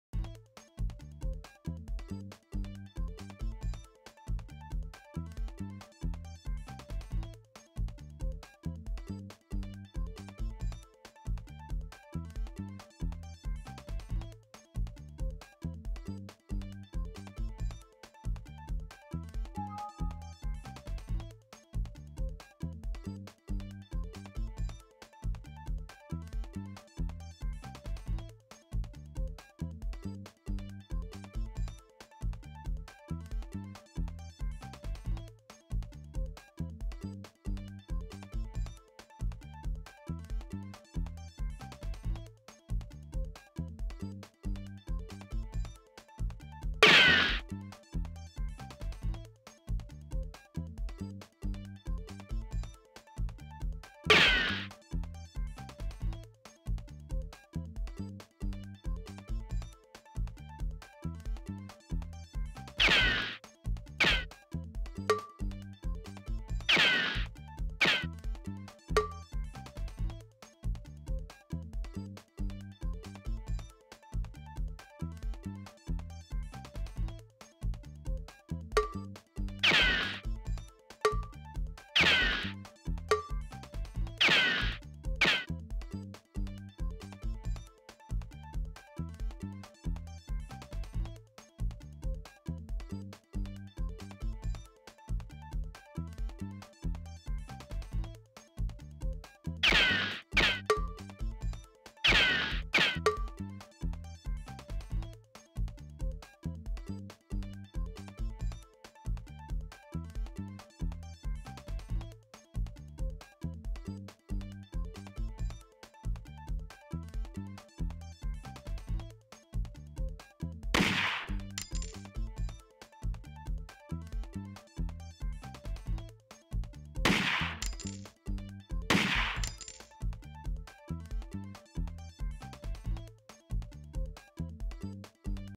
"adx2" Tutorial 2: Sequential playback in Cue (CRI Middleware)
How to use AISAC: Creating a Cue with multiple sounds, and set the sequence.